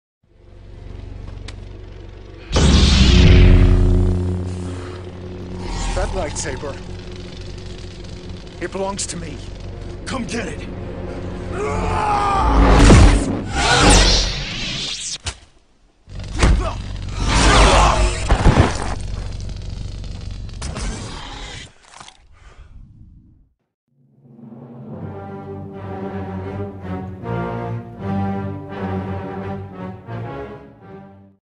Kylo Ren vs Finn (Stop Motion)
This is what should of happened during the battle between Kylo and Finn